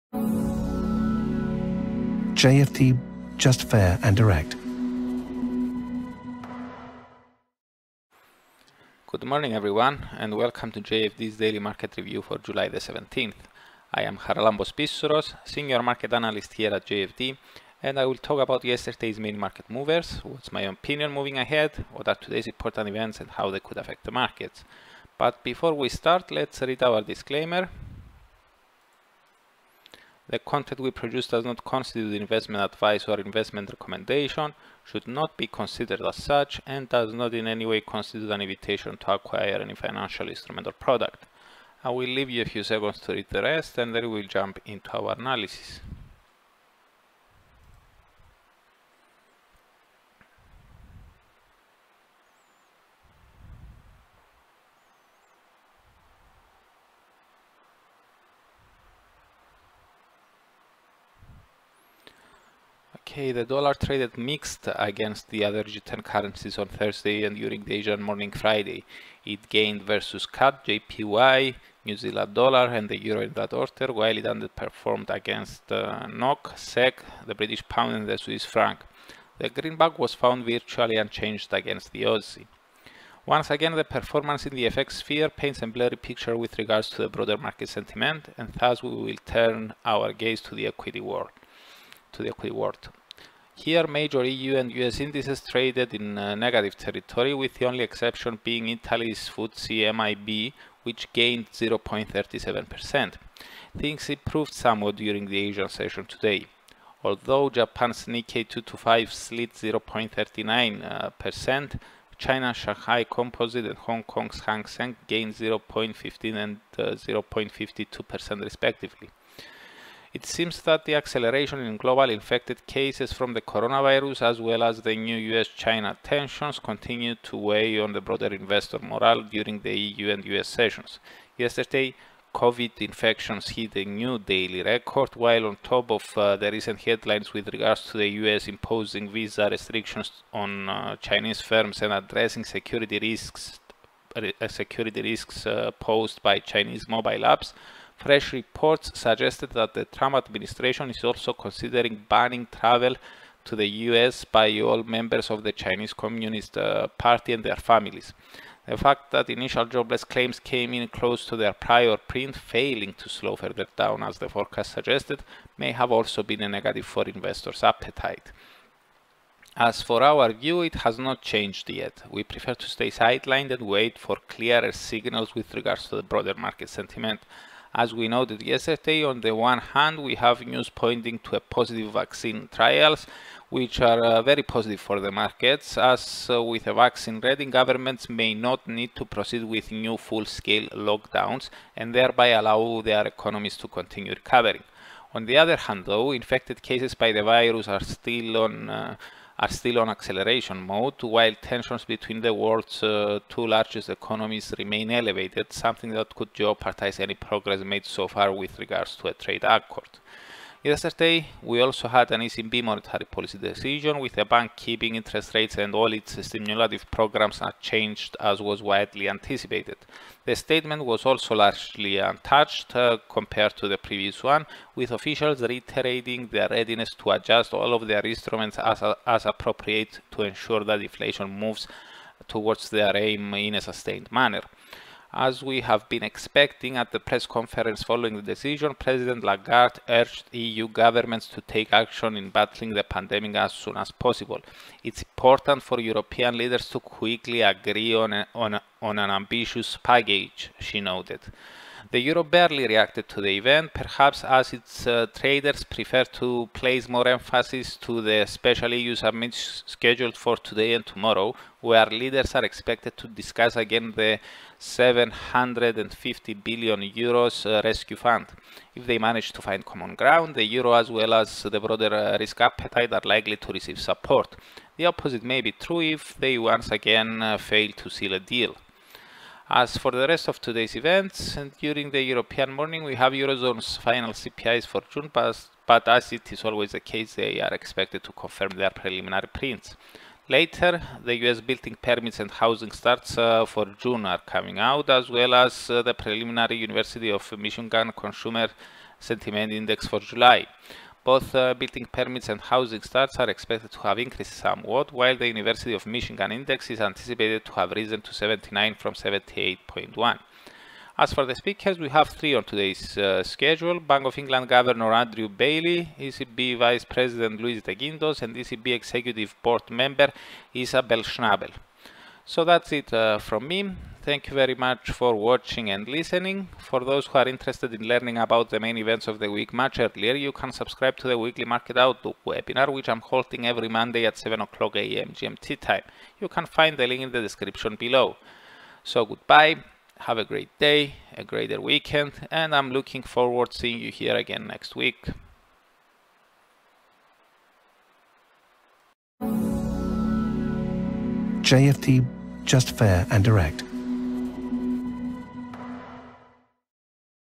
Daily Market Review: Covid-19 Cases Hit New Record, US-China Tensions Increase
Risk appetite remained subdued yesterday as global infections from the coronavirus hit a new daily record, and after reports saying that the Trump administration is considering banning travel to the US by all members of the Chinese Communist Party and their families. The ECB stood pat yesterday, with President Lagarde urging EU leaders to agree on a rescue plan at the EU summit scheduled for today and tomorrow.